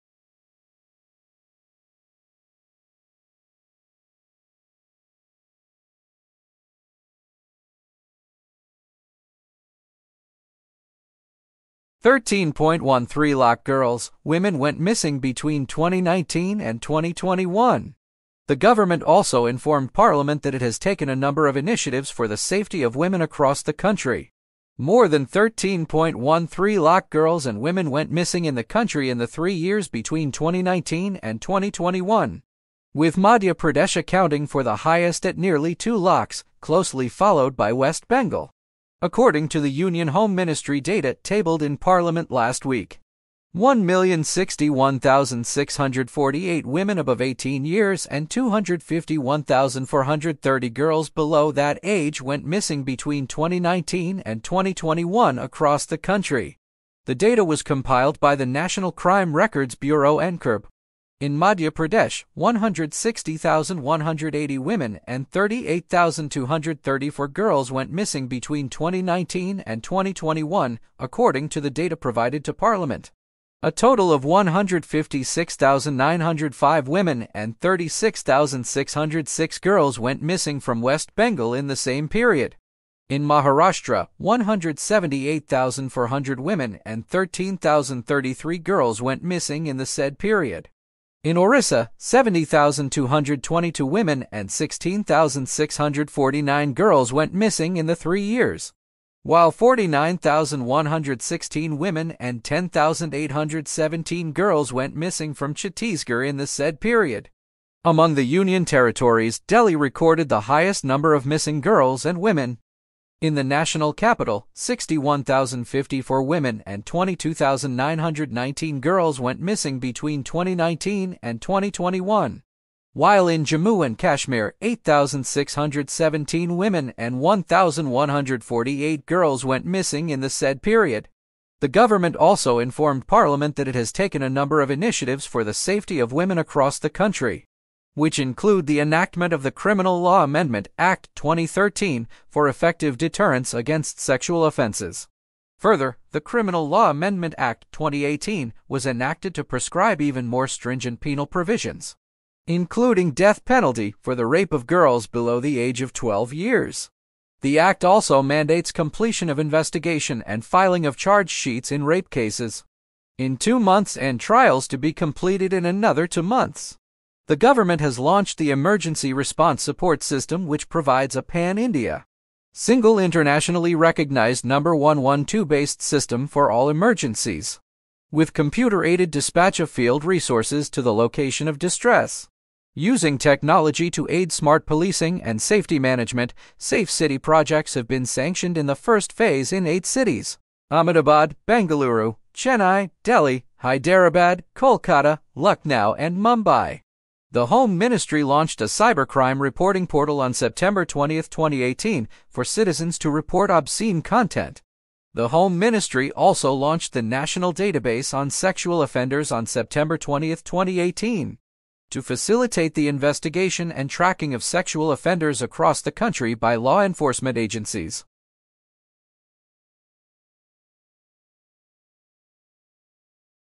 Uncovering the Mystery of 13.13 Lakh Missing Girls and Women (2019-2021)
Join us on a sobering journey into the staggering reality of the 13.13 lakh girls and women who mysteriously vanished between 2019 and 2021. In this eye-opening video, we delve deep into the statistics, exploring the harrowing stories behind these disappearances. From the heartbreak of families left searching for answers to the societal implications of this alarming trend, we confront the urgent need for awareness and action. Together, let's shine a light on the unseen and demand justice for the missing.